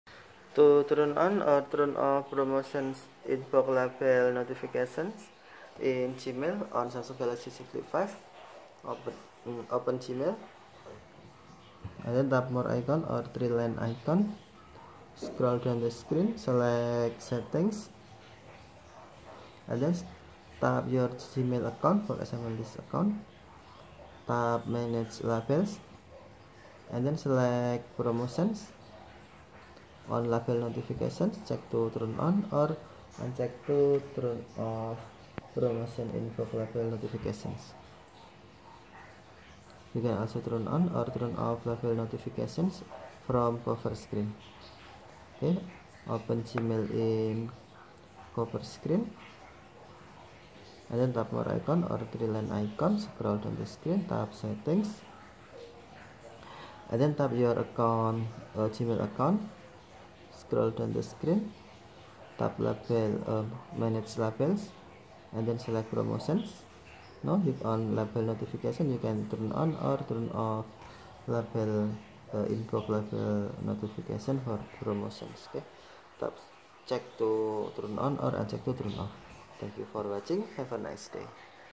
How to turn on or off Promotions Inbox Label notifications in Gmail on Android (Samsung Z Flip5)
Staying organized amidst promotional emails is essential for maintaining inbox clarity. Gmail provides the flexibility to enable or disable notifications for specific labels, including the Promotions inbox label, on your Android device. With the Samsung Galaxy Z Flip5, managing notifications for the Promotions inbox label in Gmail is straightforward. Here's how to turn them on or off:

1. **Launch Gmail App**: Unlock your Samsung Galaxy Z Flip5 and open the Gmail app from the home screen or app drawer.

2. **Access Settings**: Tap on the menu icon located in the top-left corner of the screen, often represented by three horizontal lines or dots.

3. **Navigate to Settings**: Scroll down the menu and tap on "Settings" to access Gmail settings.

4. **Choose Your Account**: If you have multiple email accounts linked to the Gmail app, select the account for which you want to manage label notifications.

5. **Select Label Settings**: Within the account settings, locate and tap on the "Label settings" or "Inbox labels" option to access label settings.

6. **Find the Promotions Inbox Label**: Scroll through the list of labels until you find the Promotions inbox label. Tap on it to access its settings.

7. **Toggle Notifications**: Within the label settings, you'll find an option to enable or disable notifications for the Promotions inbox label. Toggle the switch next to "Label notifications" to turn notifications on or off according to your preference.

8. **Adjust Other Settings** (Optional): Depending on your preferences, you may also have the option to customize other notification settings, such as sound, vibration, or notification priority. Adjust these settings as needed.

9. **Save Changes**: Once you've configured the label notification settings to your liking, tap on the "Save" or "Done" button to confirm and apply the changes.

By following these steps, you can easily turn on or off notifications for the Promotions inbox label in Gmail on your Samsung Galaxy Z Flip5, allowing you to manage your email notifications effectively and stay informed without feeling overwhelmed.